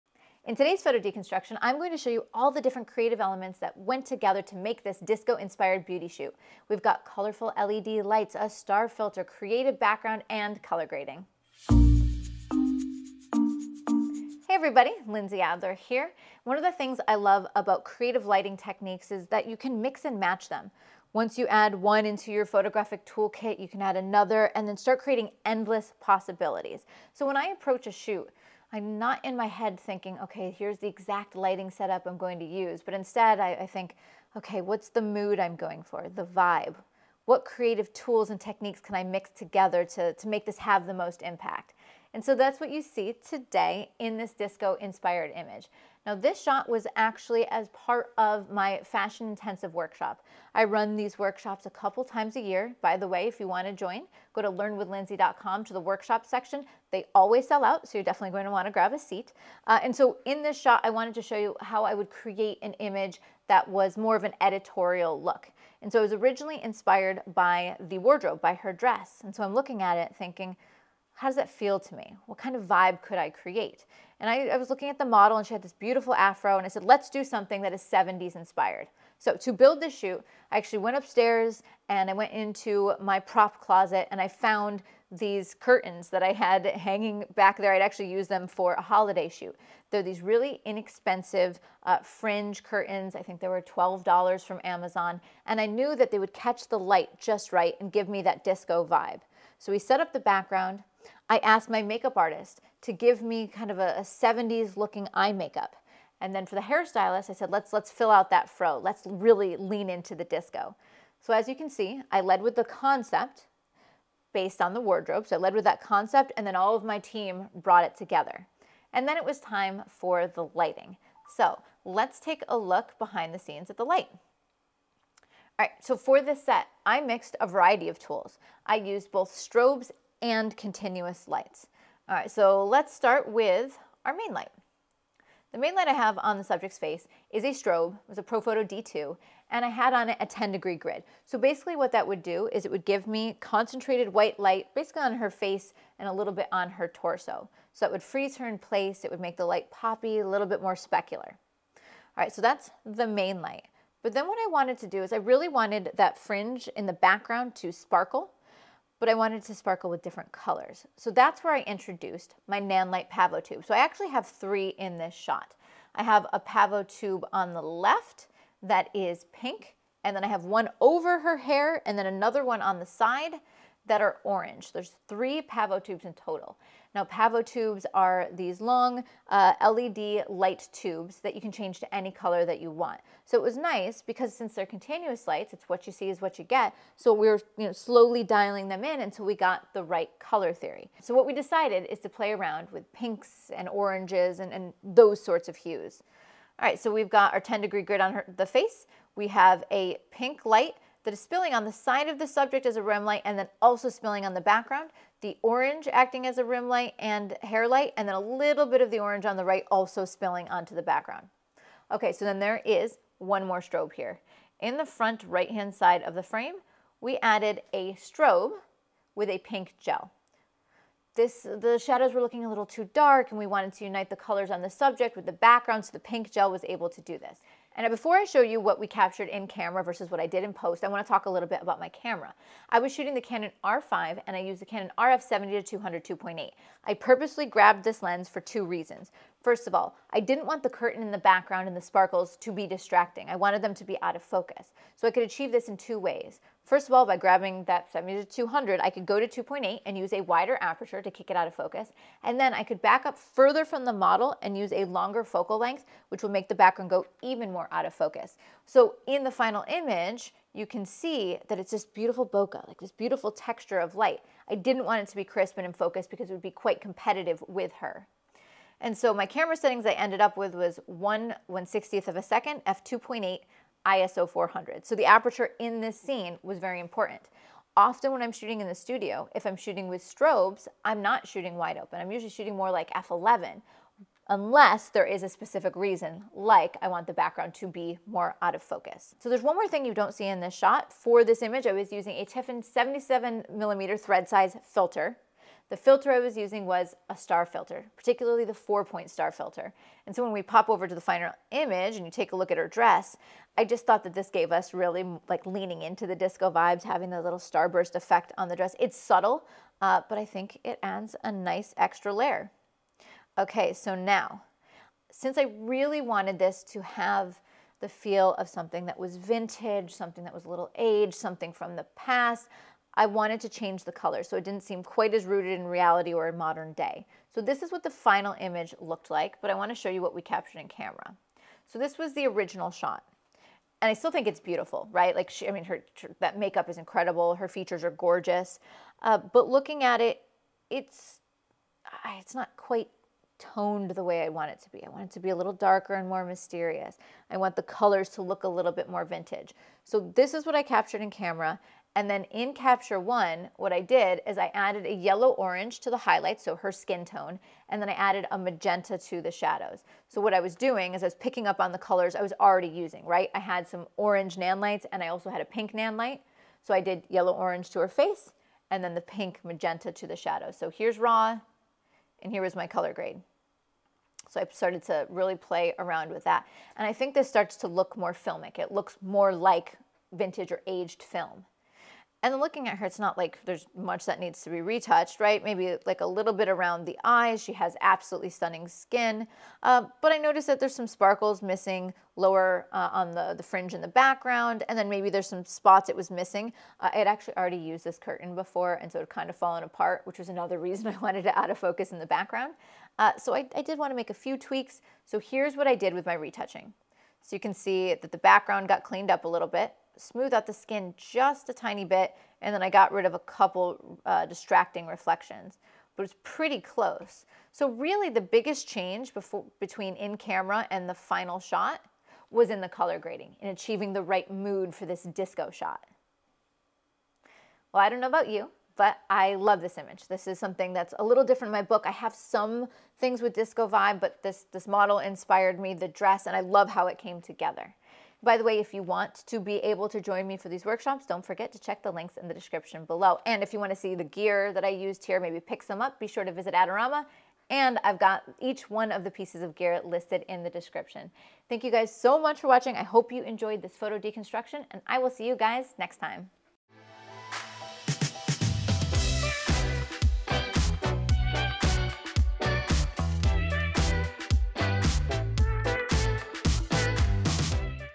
Disco-Inspired Beauty Photoshoot: Behind the Scenes with Lindsay Adler
In today's video I'll take you behind the scenes of a disco-inspired beauty photoshoot. We'll explore the creative elements, including colorful LED lights, star filters, and color grading, that came together to bring this vibrant image look to life. Plus, I'll share insights into my lighting setup and post-processing techniques to achieve the perfect mood for this retro-inspired image! 🪩

2:01 Lighting
3:53 Camera settings
5:14 Post processing
7:07 Final thoughts

💡 Lighting (5):

1. Main light: 10 degree grid
2-4. Background lights: Nanlite Pavotube x3 (orange and pink)
3. Fill Light - Bare bulb + pink gel

⚙️  Camera settings:

✅  1/160th
✅  F2.8
✅  ISO 400

📸  Gear

🔥 If you'd like to see some more lighting setups you'll definitely want to check out my Creative Studio Lighting Guide (latest version!)